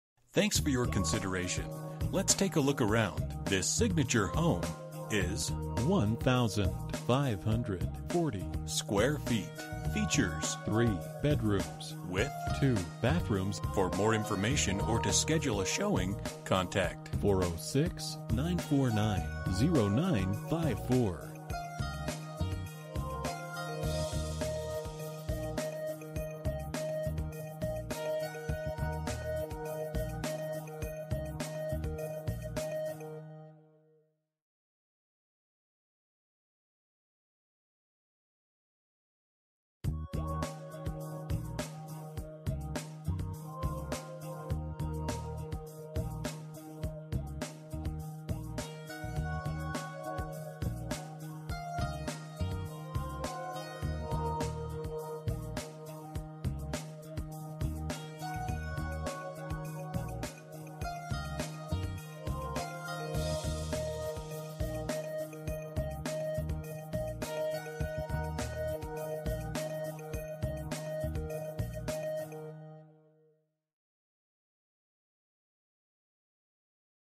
Residential for sale - 7 Rhett Drive, Townsend, MT 59644
Solid surface counters highlight this 3 BDR, 2 BTH home located in the Grandview Manor Subdivision. Beautiful views of the mountains and lake from this bright spacious home located on 1.32 acres. Single level living with a large sink in the laundry room.  The kitchen includes a pantry and a gas range.

Bedrooms: 3
Bathrooms: 2
Square Feet: 1540
Price: $274,000

For more information about this property, please contact Jeannie Steele at or . You can also text 5562485 to 67299.

05/30/2019 02:51:04 pm

Last modified: